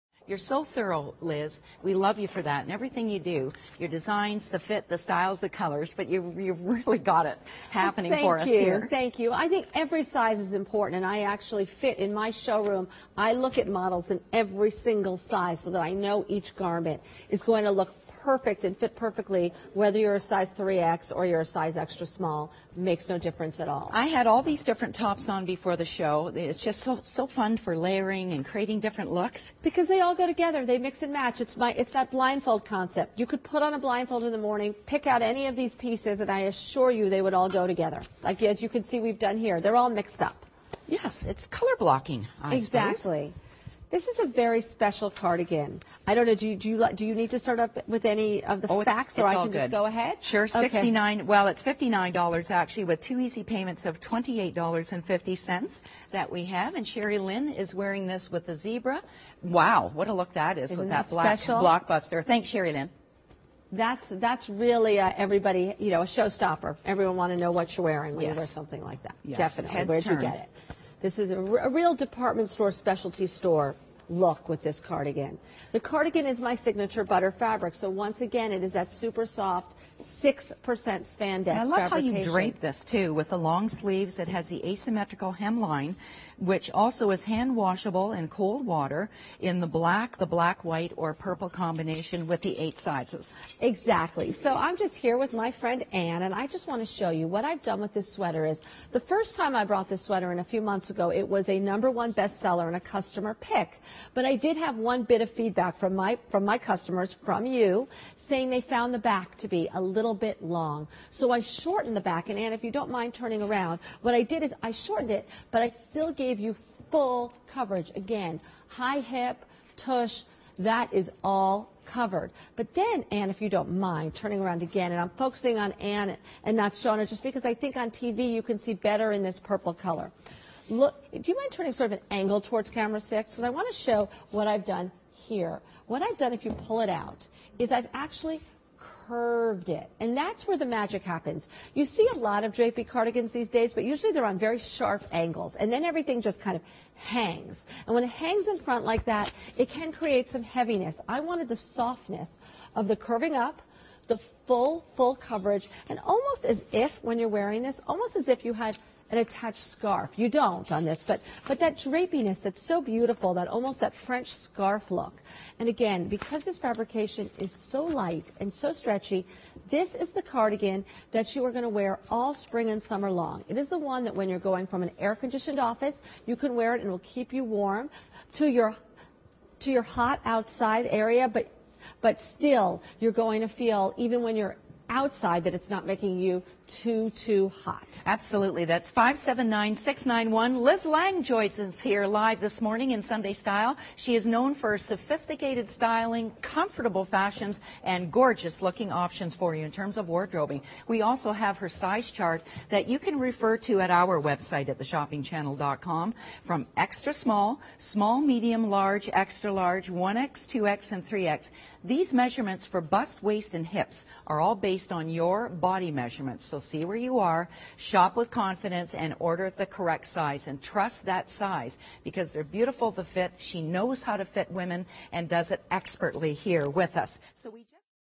Liz Lange Draped Cardigan at The Shopping Channel 579691
Totally versatile and fun to wear, this draped cardigan from the Liz Lange Collection will be in permanent rotation in your wardrobe. Whether you're looking for something to throw on over top of an outfit on a summer evening or a long sleeve cardigan to wear to the office in the middle of winter, you'll be glad you have this fabulous piece on hand.

• Long sleeve
• Asymmetrical hemline
• Fabric: 94% rayon, 6% spandex
• Care: Hand wash cold separately, do not bleach, lay flat to dry, low iron

Garment measurements in inches:
• Bust: S(33.5"), M(35.5"), L(37.5"), XL(43.5"), 1X(45.5"), 2X(49.5"), 3X(53.5")
• Sleeve length: XS(29.5"), S(30.25"), M(31"), L(31.75") XL,1X(32.5"), 2X(33.25"), 3X(34")
• Sweep: XS(40.5"), S(42.5"), M(44.5"), L(47.5"), XL(50.5"), 1X(53"), 2X(57"), 3X(61")
• Length: XS(27.5"), S(28.25"), M(29"), L(29.75"), XL(30.5"), 1X(30.5"), 2X(31.25"), 3X(32")